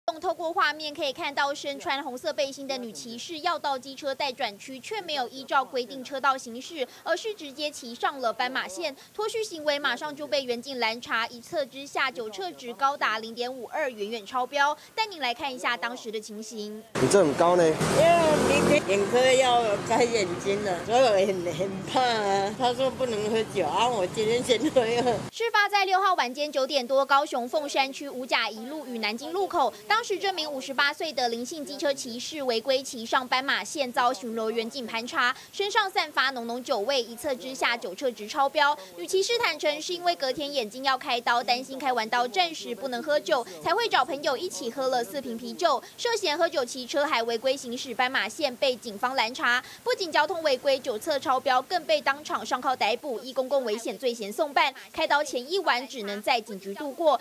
稱「怕開刀後不能喝」 女酒駕上路遭逮【最新快訊】
高雄一名林姓女騎士，6號晚間9點多，騎車行經鳳山區 五甲一路與南京路口，違騎斑馬線被員警攔查，一查發現酒測值超標，女騎士才坦承，是因為隔天眼睛要動手術，擔心術後暫時不能喝酒，才會在開刀前找朋友小酌幾杯，詳細情況連線記者 蘇瑜玟。